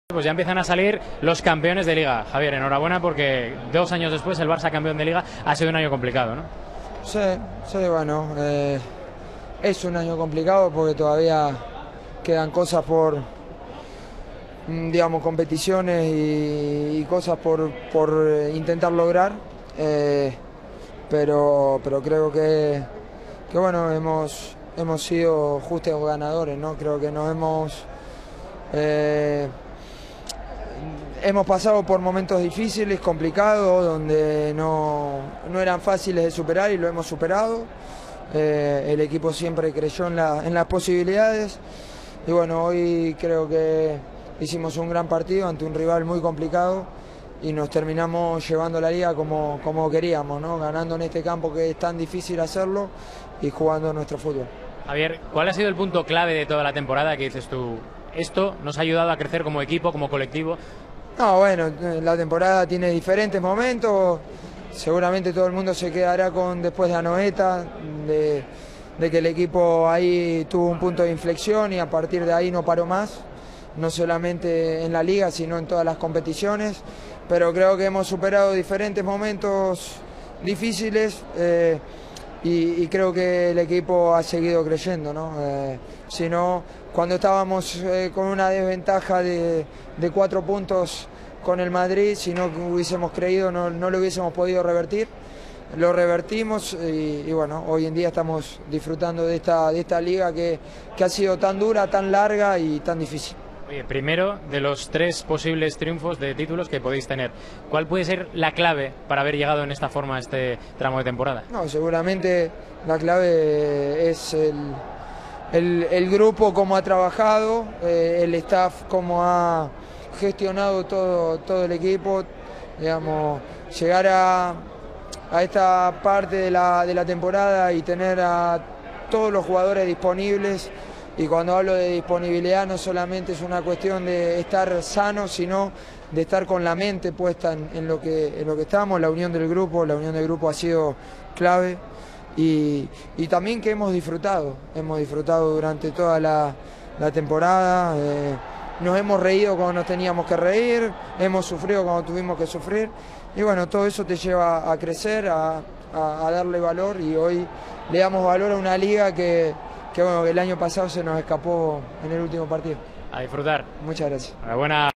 Mascherano: "El equipo ha superado momentos difíciles y ha sabido crecer"
Mascherano: "El equipo ha superado momentos difíciles y ha sabido crecer" SUSCRIBETE AL MEJOR CANAL HD La Liga | Entrevista a Mascherano tras el Atlético de Madrid (0-1) FC Barcelona J37 Liga BBVA Youtube